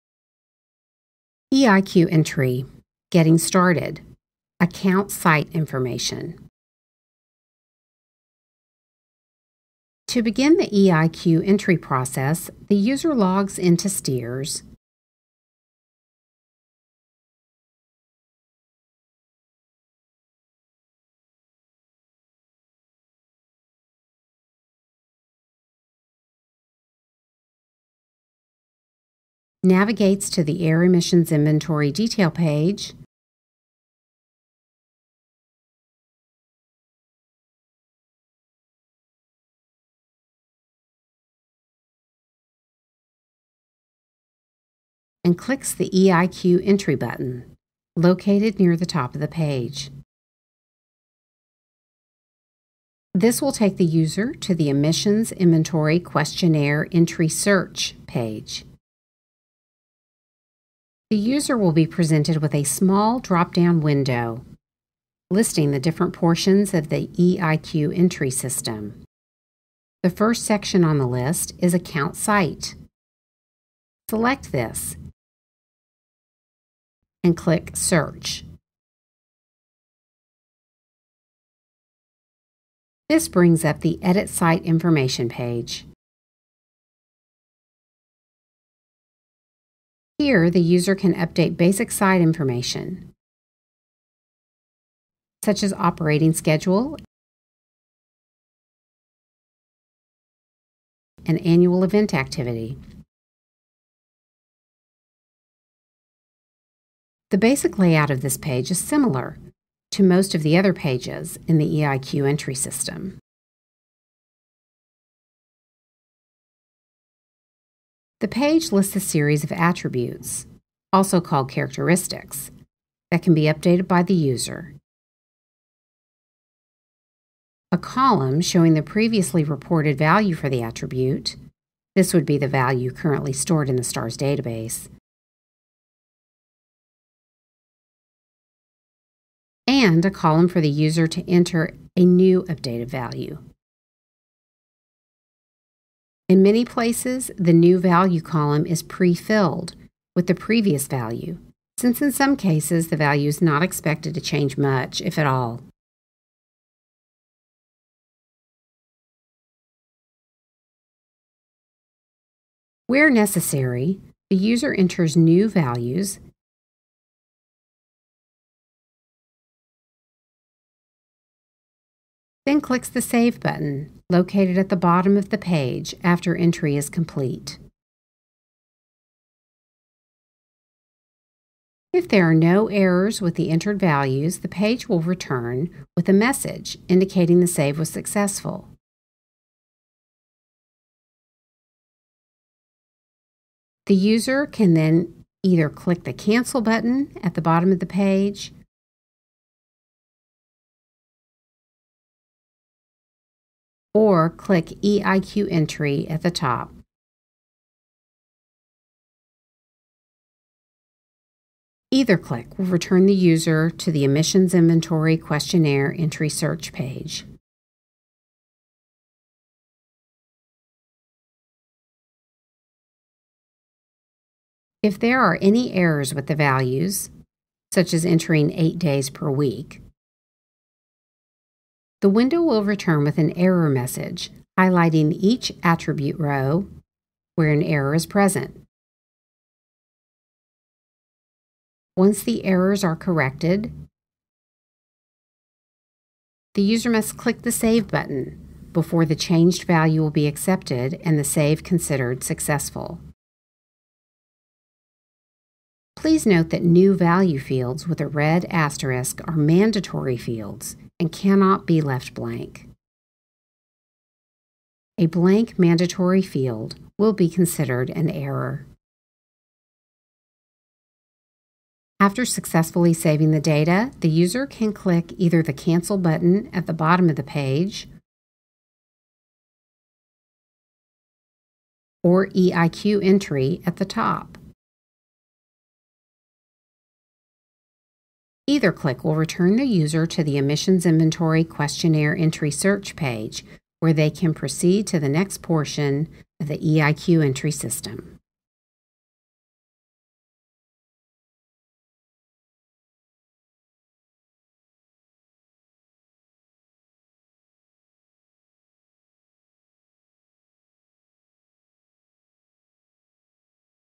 Web-EI 4. EIQ Entry – Getting Started: ACCOUNT-SITE Information
Learn about the EIQ entry option for submitting an emissions inventory (EI) through the Annual Emissions Inventory Report (AEIR) Web-EI system in the State of Texas Environmental Electronic Reporting System (STEERS). This video shows how to edit site information and provides an overview of basic EIQ entry functions.